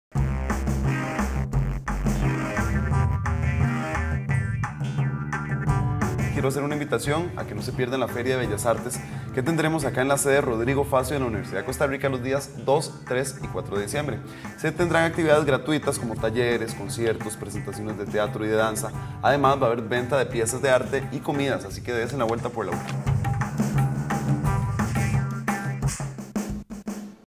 Feria de Bellas Artes en la UCR
Se viene la Feria de Bellas Artes en la Universidad de Costa Rica. Del 2 al 4 de diciembre,  se ofrecerán actividades gratuitas como talleres, conciertos, presentaciones de teatro y danza. Además, venta de piezas de arte y comidas.

EL HORARIO:
Viernes 2 y sábado 3 de diciembre de 10:00 a.m. a 7:00 p.m. Domingo 4 de diciembre de 10:00 a.m. a 6:00 p.m.